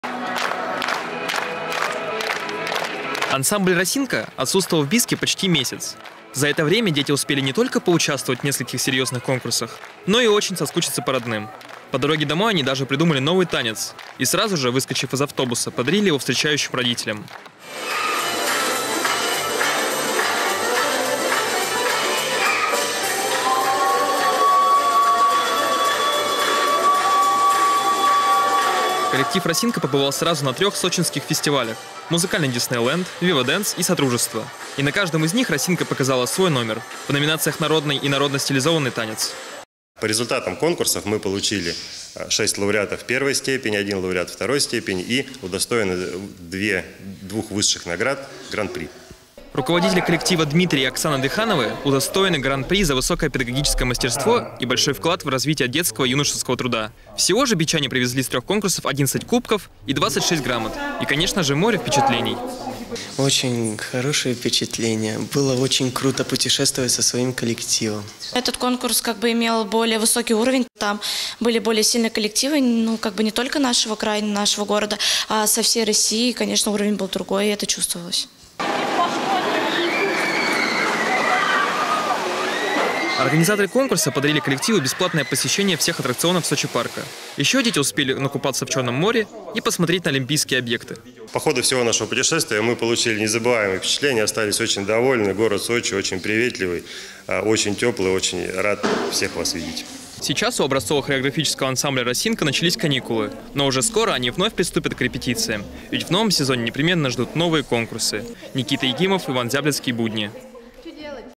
"Хореографический ансамбль "Росинка" вернулся из Сочи" (Будни, 19.07.16г., Бийское телевидение)
Очередная творческая победа. Хореографический ансамбль "Росинка" принял участие сразу в трёх сочинских фестивалях. В родной город бийчане привезли 11 кубков и 26 грамот.